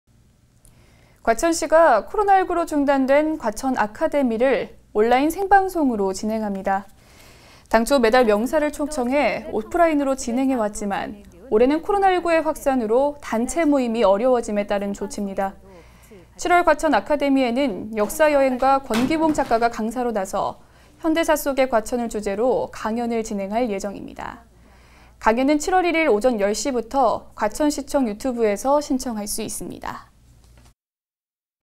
[과천] 7월 과천아카데미, 1일 온라인 생중계
과천시가 코로나19로 중단된 과천아카데미를
온라인 생방송으로 진행합니다.

당초 매달 명사를 초청해 오프라인으로 진행해왔지만
올해는 코로나19의 확산으로
단체 모임이 어려워짐에 따른 조치입니다.

7월 과천아카데미에는
역사여행가 권기봉 작가가 강사로 나서
'현대사 속의 과천'을 주제로
강연을 진행할 예정입니다.

강연은 7월 1일 오전 10시부터
과천시청 유튜브에서 시청할 수 있습니다.